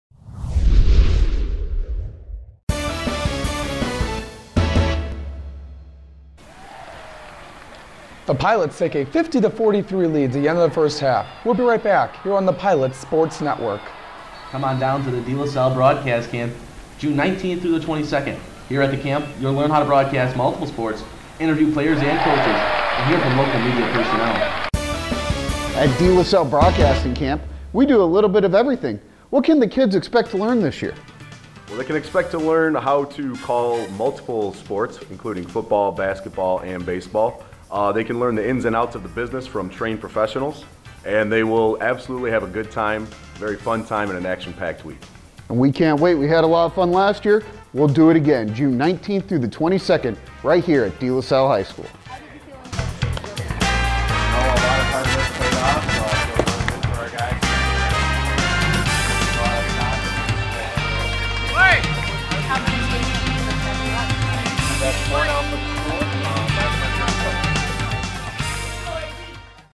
De La Salle Collegiate Sports Broadcasting Camp 2017 Preview
Do you love sports? Would you enjoy a career as a TV sportscaster? Have you dreamed about being a TV sports anchor or calling a last second, game winning shot on radio or television? Then, this is your chance to learn how to become a sports broadcaster!

At the De La Salle Sports Broadcasting, students will learn the basics of putting together a sports broadcast including: Play-by-play announcing techniques, color commentary, player and coach interviews, and technical use of equipment. Campers will have an opportunity to call live-action scrimmages provided by DLS athletic camps. Students will also get the opportunity to visit with industry professionals from the Detroit Sports market, as they share their insight and advice for aspiring broadcasters. Past guests have included radio/television personalities Trevor Thompson from Fox Sports Detroit and Jeff Riger from 97.1 The Ticket.